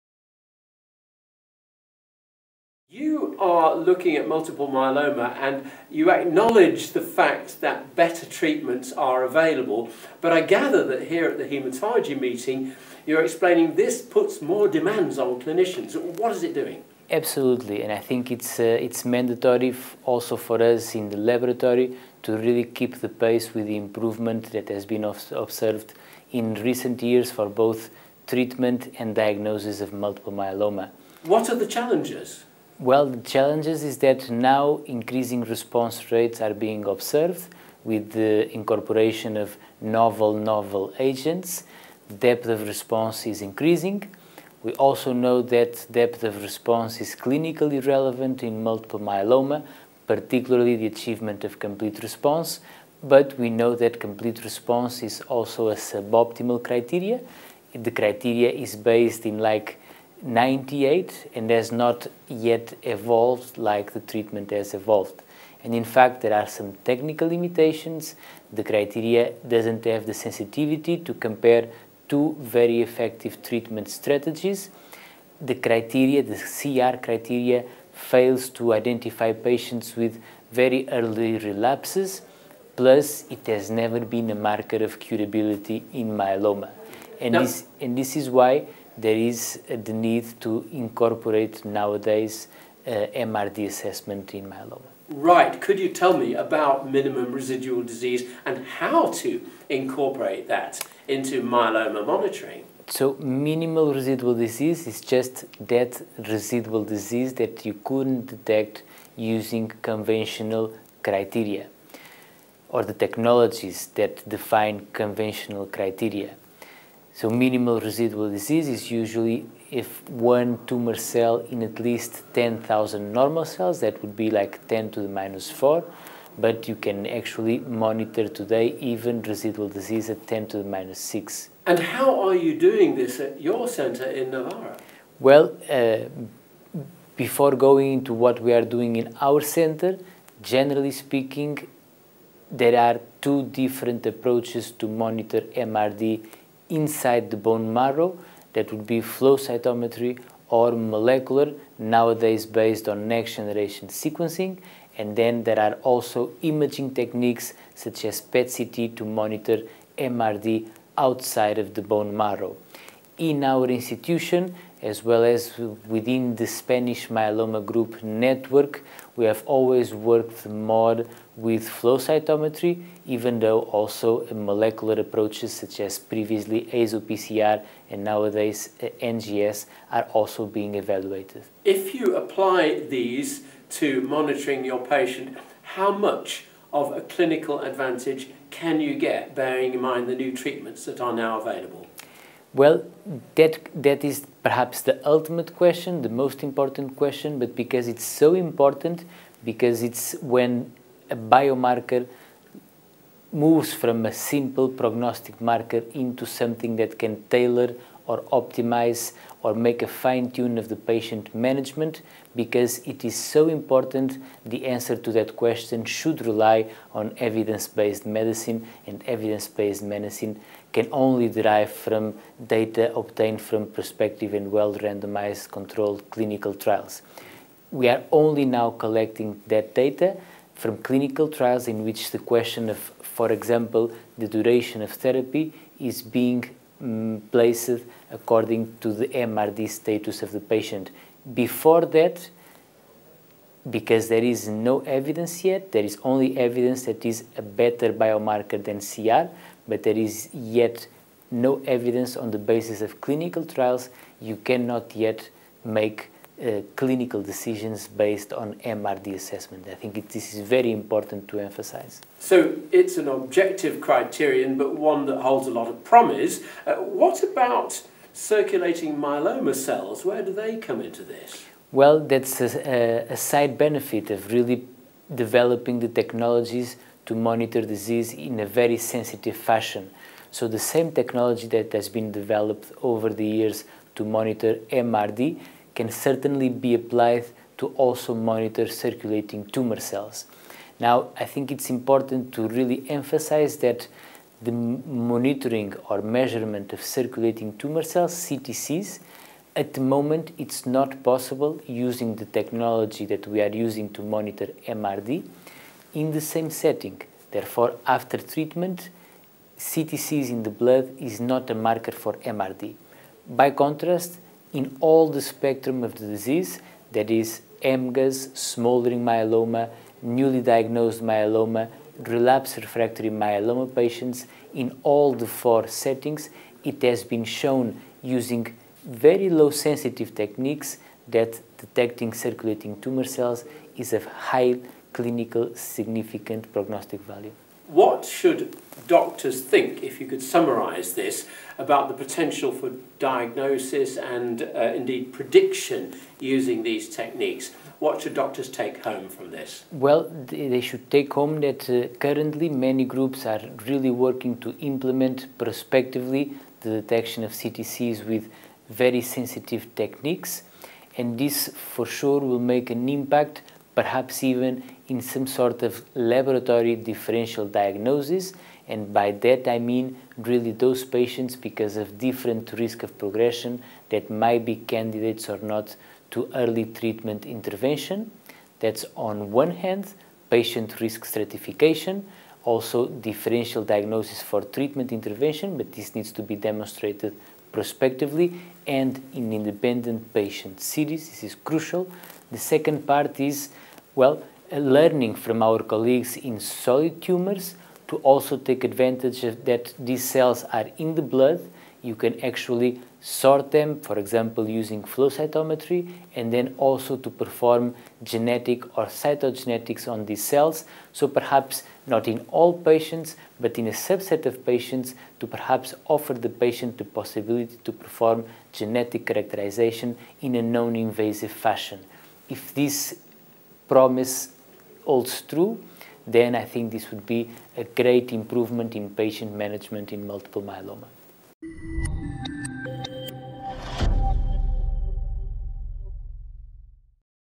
Minimal residual disease in multiple myeloma
Dr Paiva (University Clinic of Navarra, Pamplona, Spain) talks to ecancertv at EHA 2015 about the need to better monitor minimal residual disease in multiple myeloma.